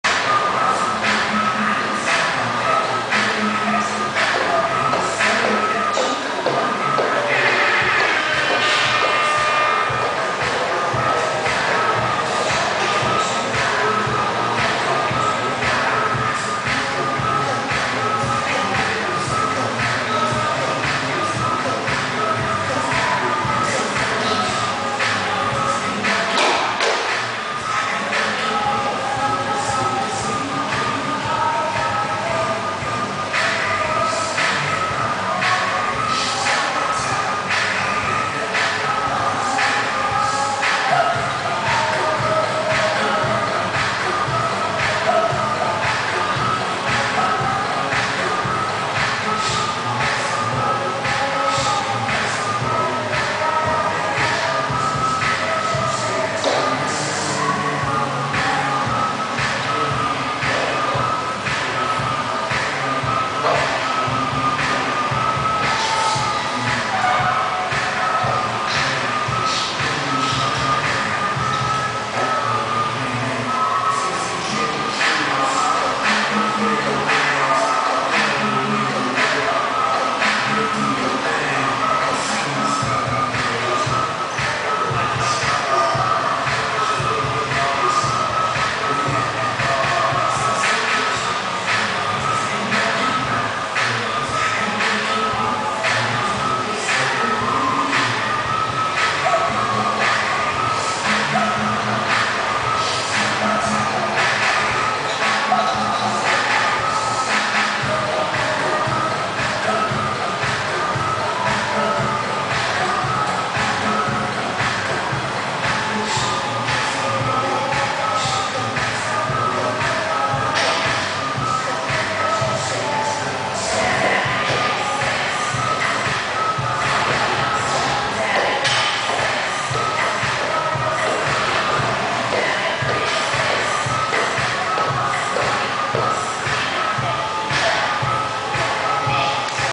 Pole Kat @ Goddess A GoGo
playing around at Goddess before the workshop with Karol